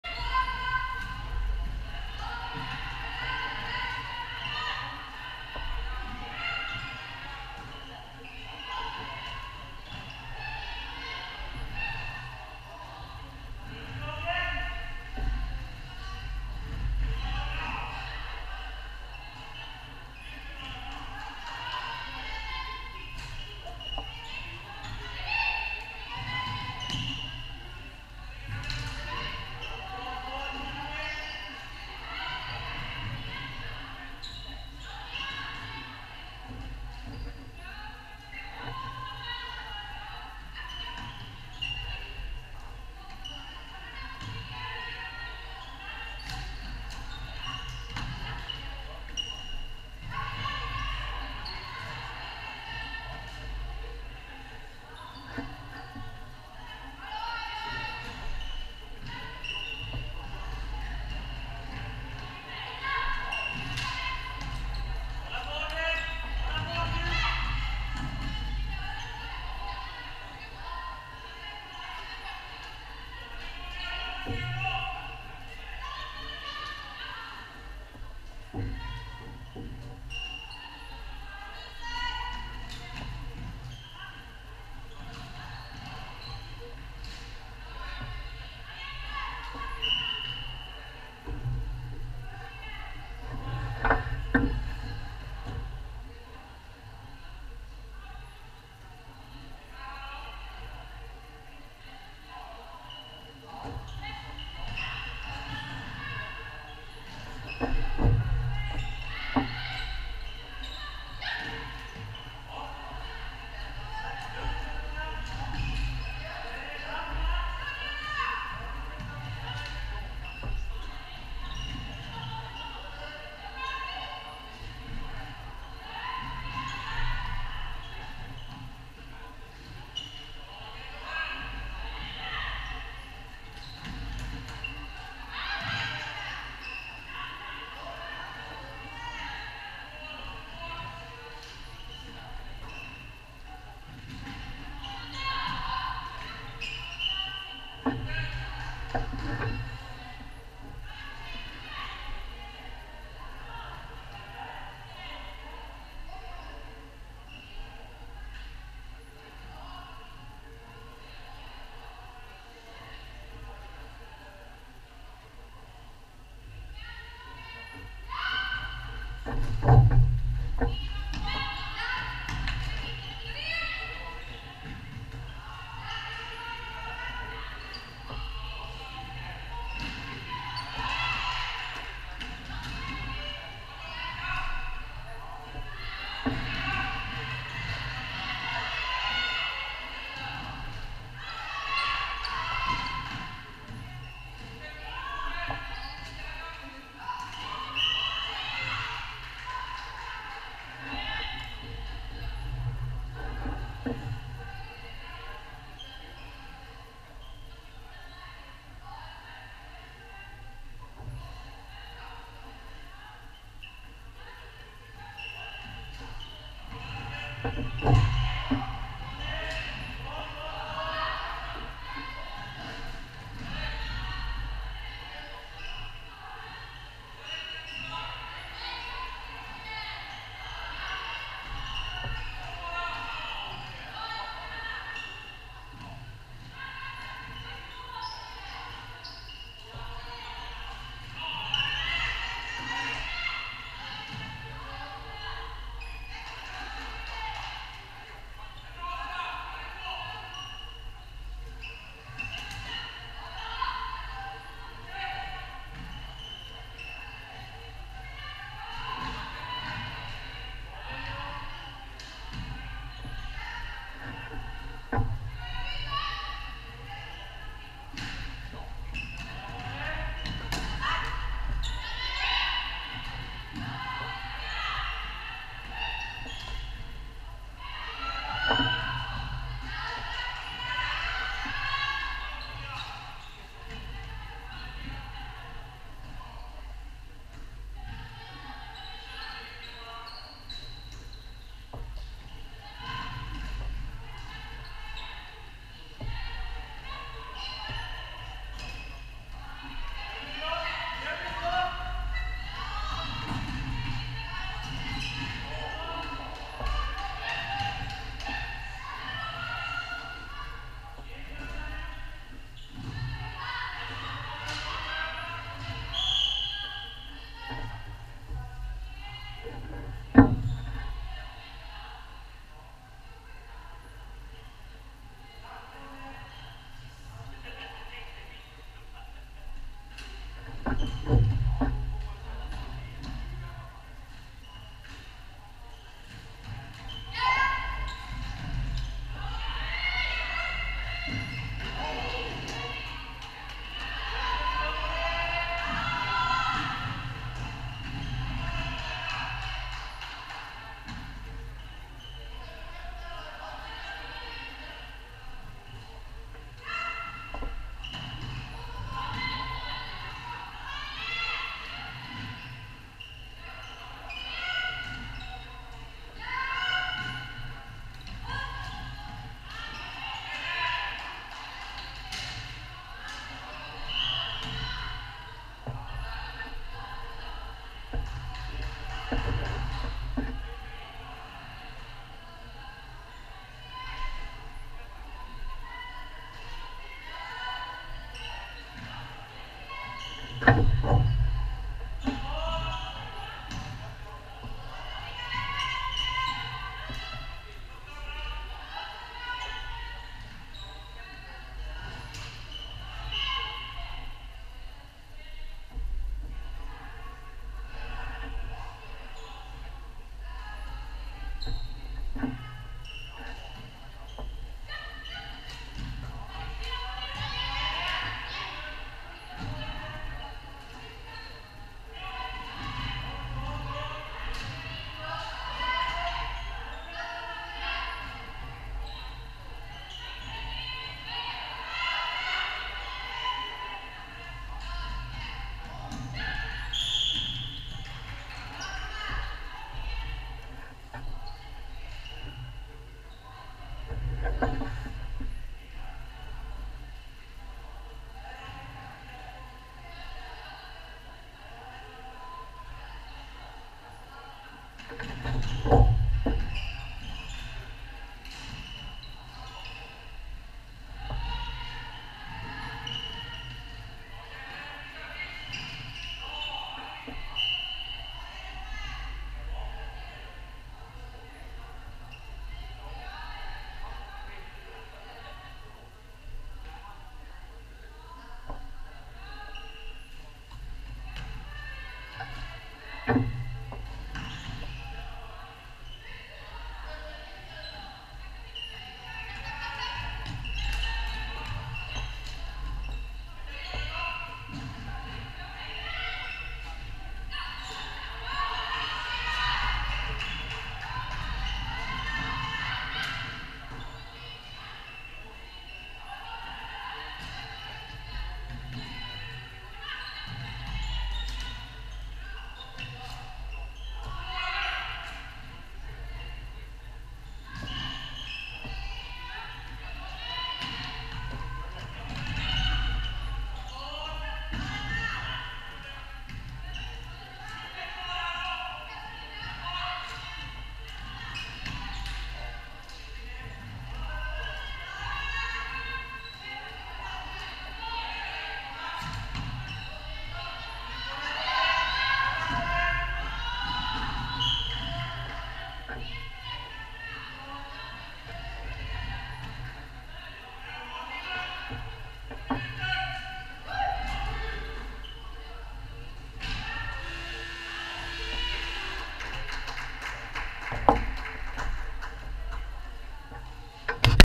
170226 GOPRO Damer Div.1 Lindås IBK - IBK Walkesborg 99 (7-2) HD Per1B
Lindås Damlag Div1
Ledare;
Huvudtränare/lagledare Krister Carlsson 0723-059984
Assisterande tränare Martin Ringius
Målvakter
94 Jonna Magnemyr 2000
95 Gisela Jägebratt 1995
Utespelare
17 Johanna Börjesson 1992
5 Johanna Hansson 1994
25 Ingrid Svensson 1994
2 Teodora Berndtsson 1998
15 Sigrid Persson 1993
6 Ida Börjesson 1991
21 Josefin Torell Höjesjö 1998
18 Eira Torbrand 1998
12 Emma Wessberg 1995
19 Ellen Norrfjärd 1994
88 Ebba Långelid 1997
8 Eira Göransson 1997
28 Evelina Eklind 1990
45 Christin Gundersen 1998
88 Isabelle Korhonen 1998
51 Amanda Hagquist 1998
82 Carolina Berntsson 1998
4 Sandra Ekdahl 1987
18 Sofia Ericsson 1998
66 Jennifer Johansson 1994
42 Elin Hagström 2000
89 Gabriella Hjält 1987
74 Ida Bergenhus 1994
9 Sandra Knookala 1989
7 Amanda Lindqvist 1994
92 Nathalie Lindgren 1999
80 Stina Dellborg 1998